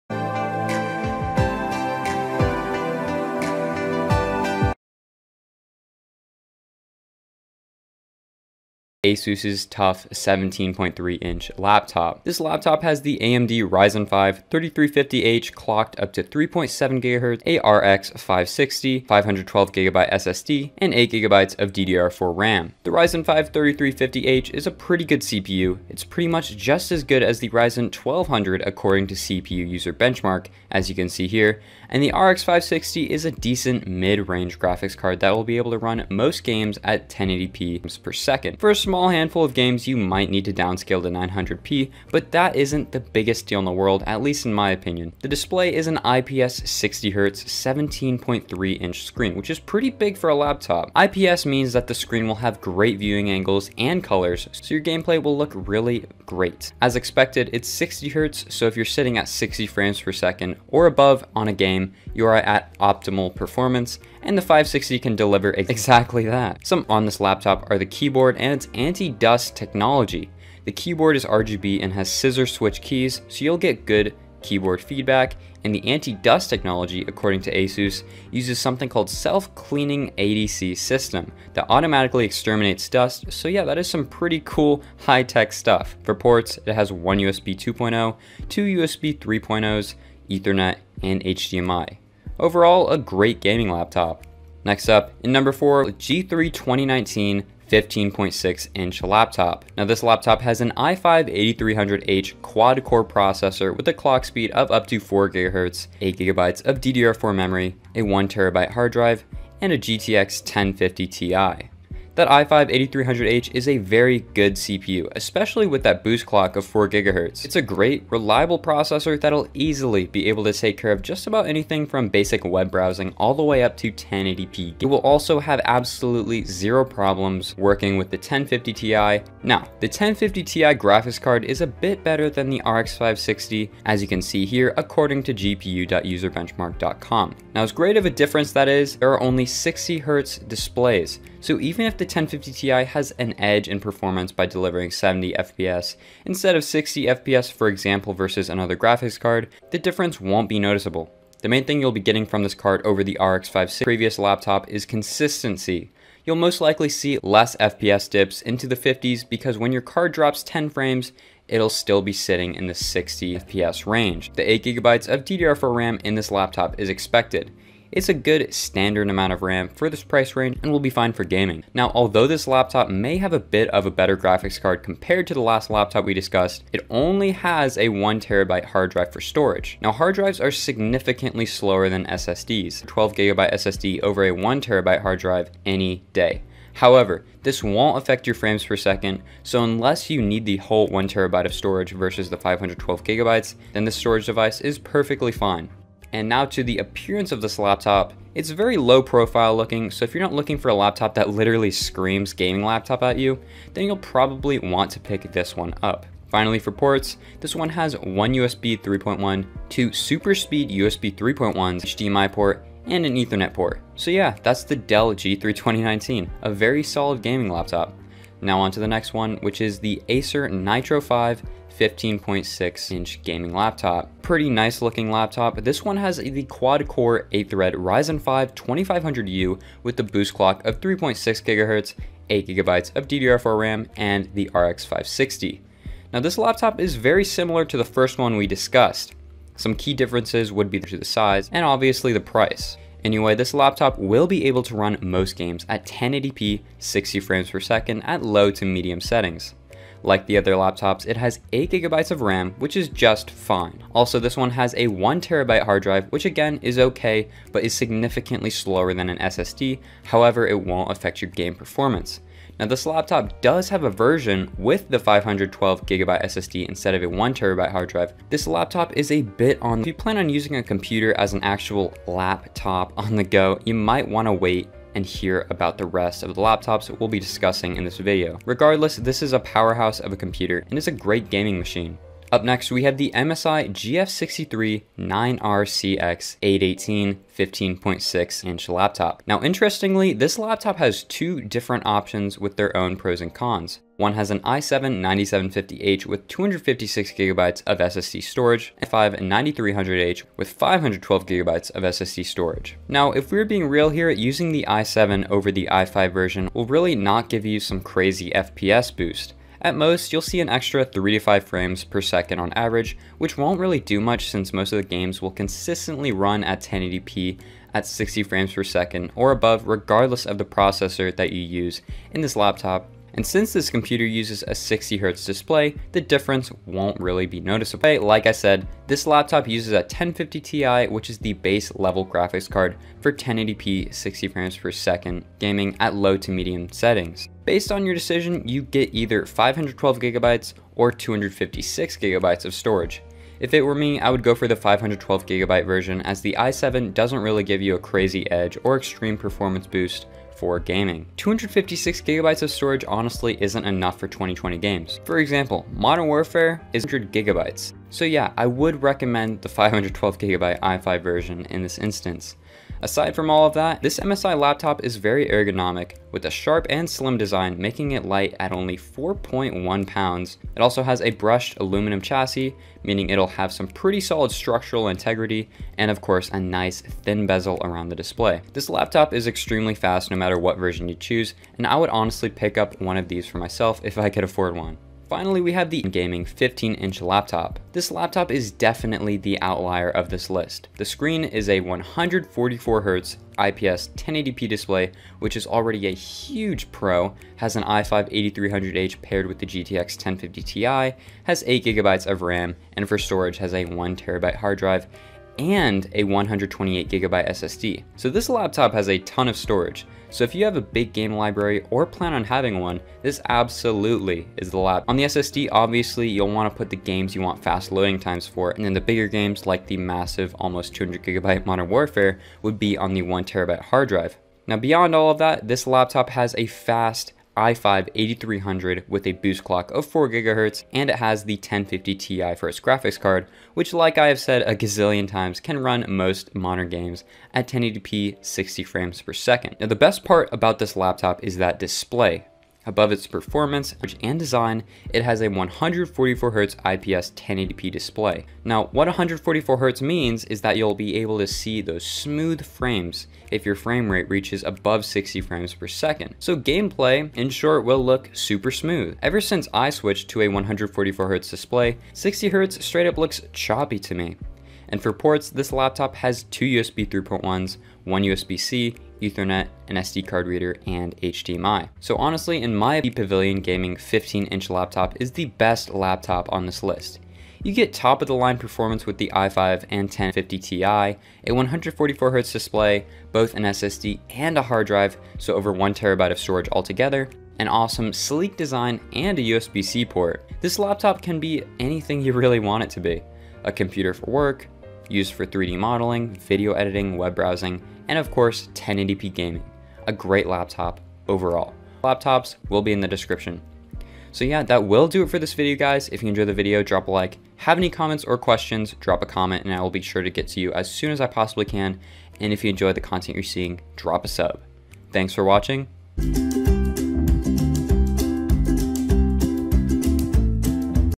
5 Best Gaming Laptops Under $800 In 2021 | E Gadgets Mania
5 Best Gaming Laptops Under $800 In 2021

Here are the links to the Products you can buy on Amazon:

5) ASUS TUF 17.3 inch Laptop

4) Dell G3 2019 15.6" Laptop

3) 2020 Acer Nitro 5 15.6" rx560

2) MSI GF63 THIN 9RCX-818 15.6" ,1050ti, i7

1) HP Pavilion Gaming 15-inch Laptop